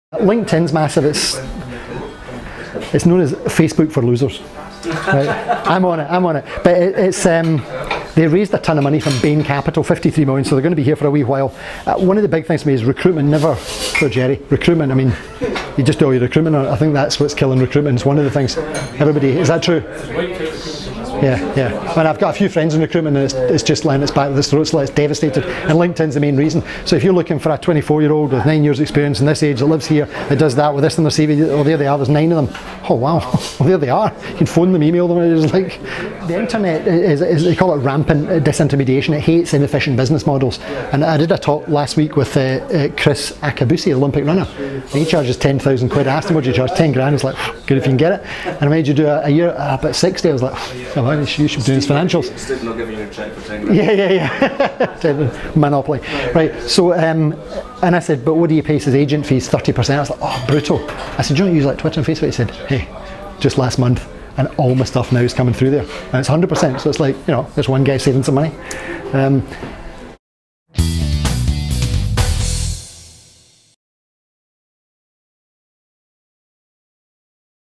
Stephen Whitelaw on the impact of LinkedIn on the recruitment industry (Part 31 of 35)
talks The future of the Web.